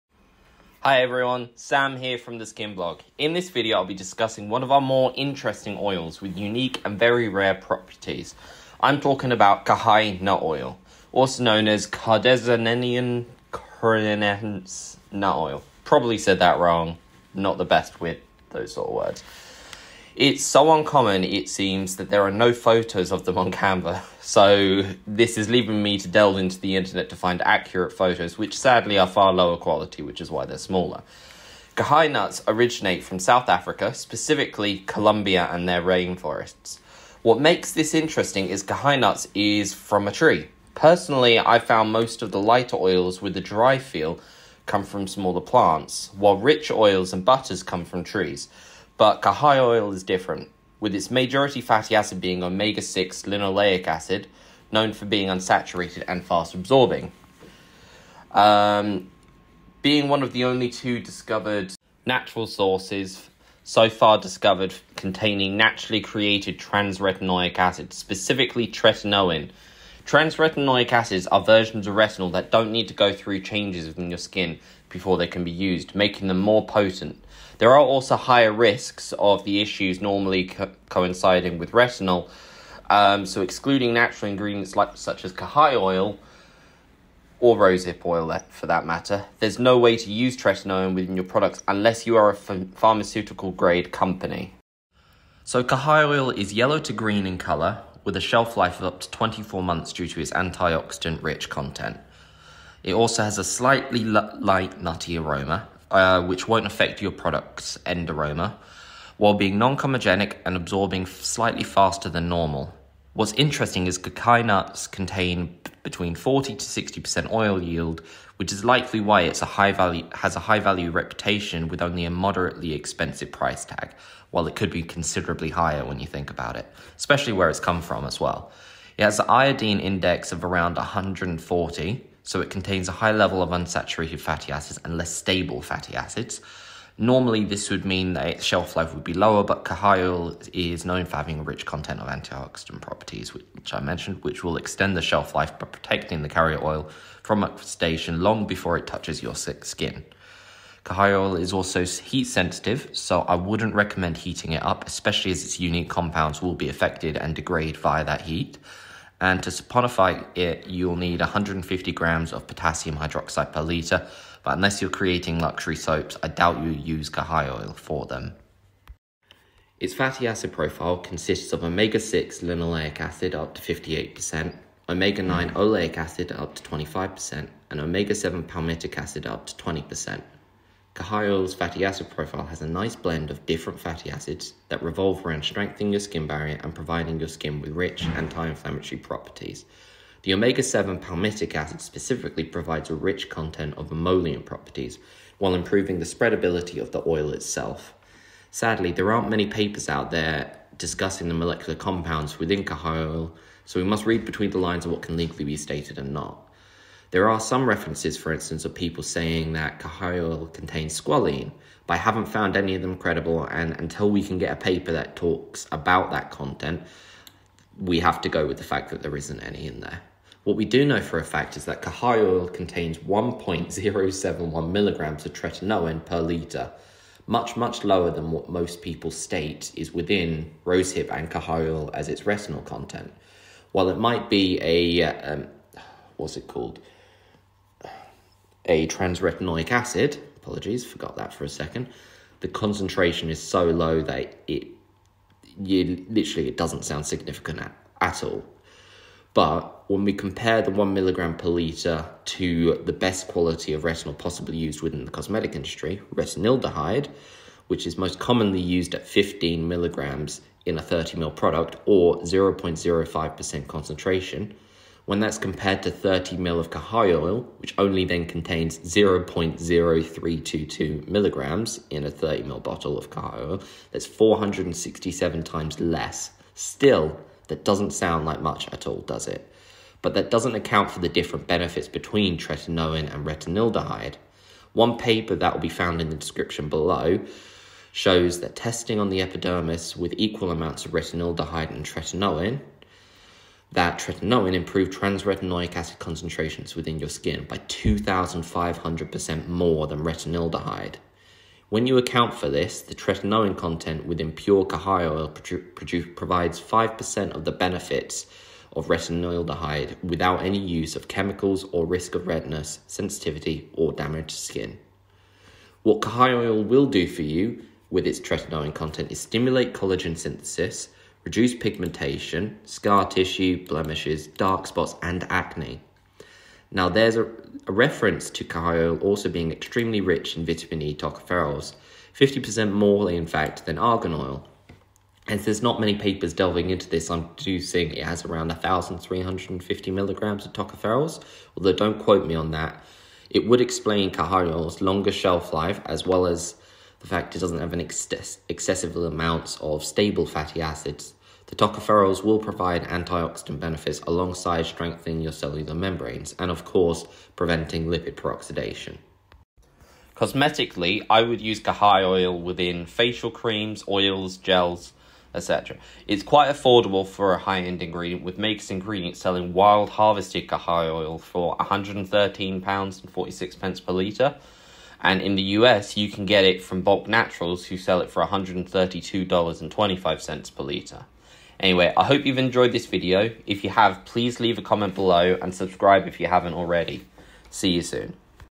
Cacay, Kahai Nut Oil, INCI: Caryodendron Orinocense Nut Oil. Vitamin A, Retinol, Tretinoin Rich.
Cacay Nut Oil, Cosmetic Profile:

INCI Name: Caryodendron Orinocense Nut Oil

Shelf Life: 24 Months

Colour: Yellow - Green

Scent: Light Nutty Aroma

Extraction Method: Cold Pressed

Comedogenic Level: 2

Absorption Rate: 2

Skin Benefits: Hydrating, Increased Cellular Turnover, Antioxidant, Anti Inflammatory, Improve Water Retention, Moisturising, Cellular Regeneration, Collagen Synthesis, Improve Elasticity, Regulate Sebum Levels

Improves: All Skin Types, Acne, Acne Scars, Hyper Pigmentation, Blemishes, Eczema, Psoriasis, Dark Spots

Molecular Profile: Omega 6: Linoleic Acid 58%, Omega 9: Oleic Acid 25%, Omega 7: Palmitic Acid 20%

Chemical Composition: Trans Retinoic Acid / Tretinoin (3 times more than Rosehip), Iodine, Plant Sterols

Minerals: Iron, Phosphorus, Potassium

Vitamins: A (Trans Retinoic Acid), B1 (Thiamine), B2 (Riboflavin), E (Alpha, Beta, Gamma, Delta Tocopherol)

Nut Oil Content: 40-60%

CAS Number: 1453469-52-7

EINECS Number: N/A

Iodine Index: 140

Saponification Value: 150mg KOH / Gram